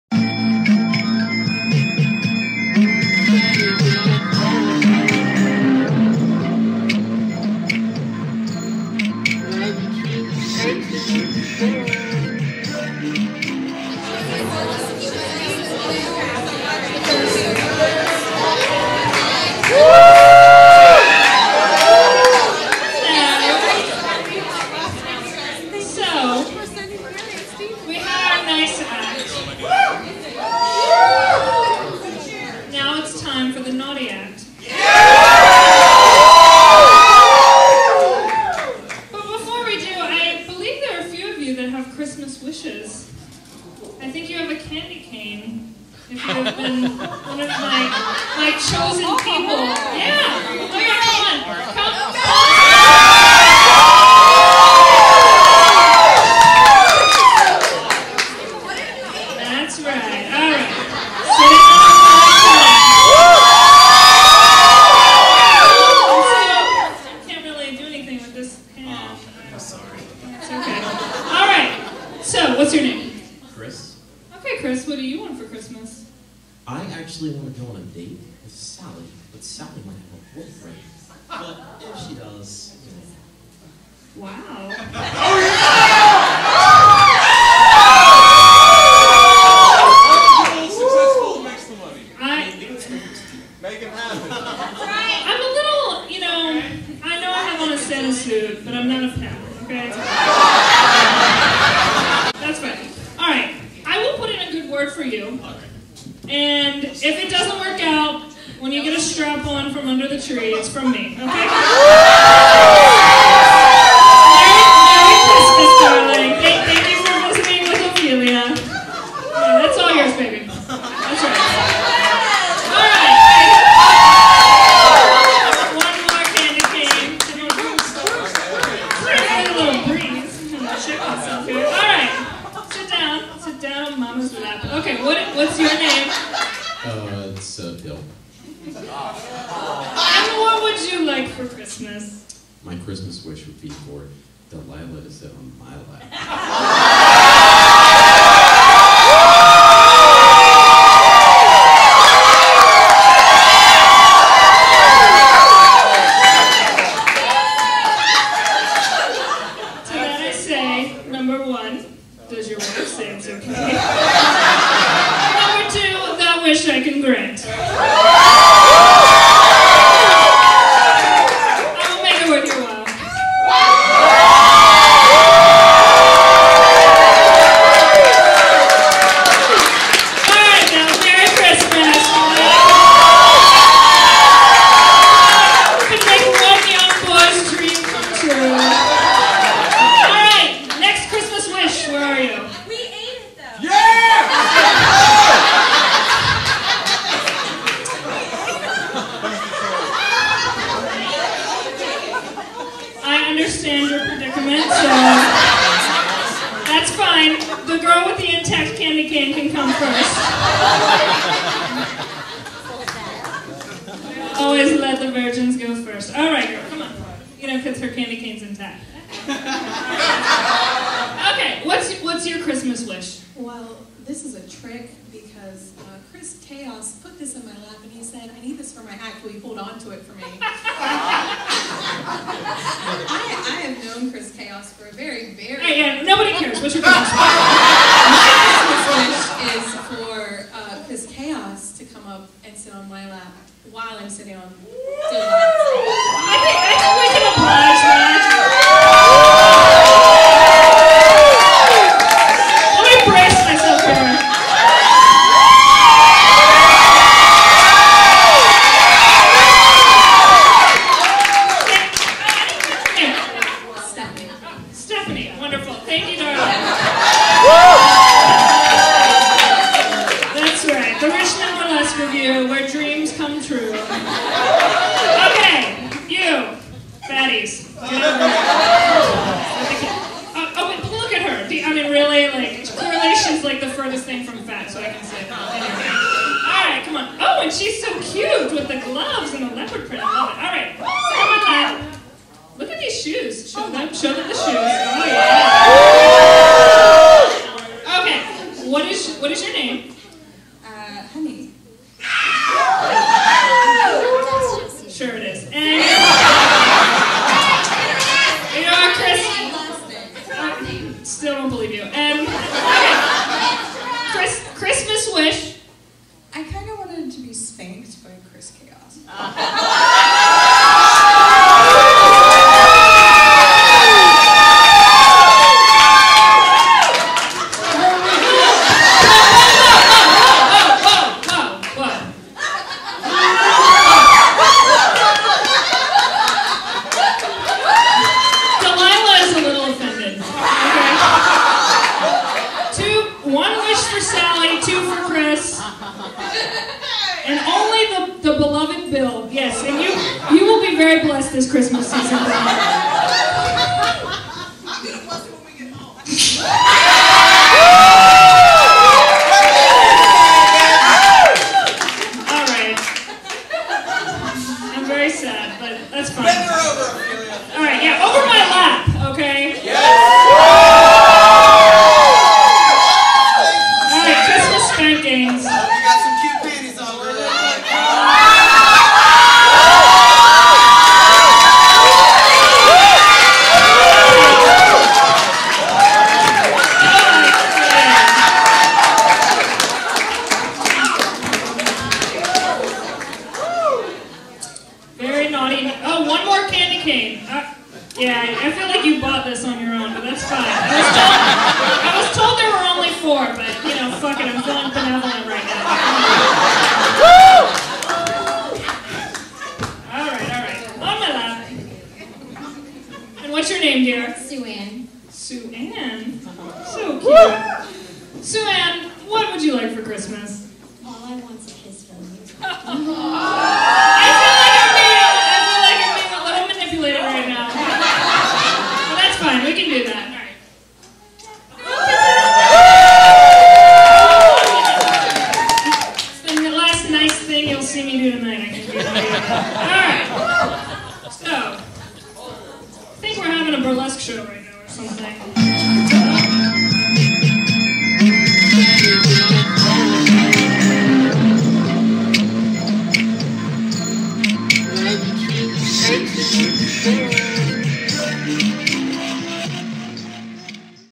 Part 8/15 - Xmas Wishes w/Ophelia - 2012 Dec. 1 "Naughty & Nice" Burlesque Revue - Richmond, VA
Ophelia invited audience members onstage to sit on her lap and tell her what they wanted for Christmas.

"Naughty and Nice" was a Christmas themed burlesque show presented by the Richmond Burlesque Revue at Gallery5 in Richmond, Virginia.